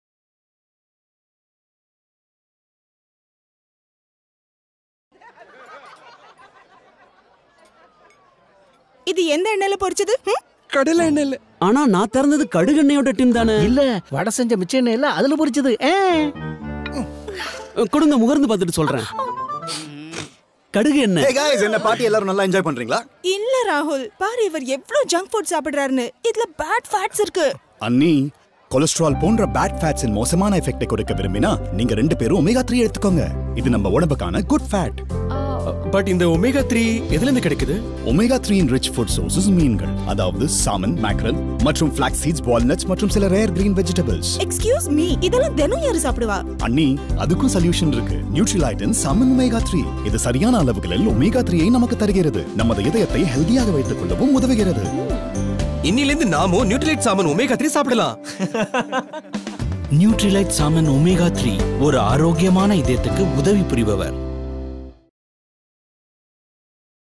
Nutrilite Salmon Omega-3 for your Heart health | Tamil
Interesting & funny 1 minute video around a couple’s conversation at a party scene, revolving around a ‘conscious wife’ concern over her husband’s indulgence with fried foods at party Through couple’s conversation, video establishes science behind ‘Good fats & Bad fats’ in easy to understand language. Along with ‘food items rich in Omega 3’ as well as ‘Deficiency of Omega 3’ in Indian diets. Video ends with Salmon omega 3  benefits for Heart health. Entire Story narrated in 1 minute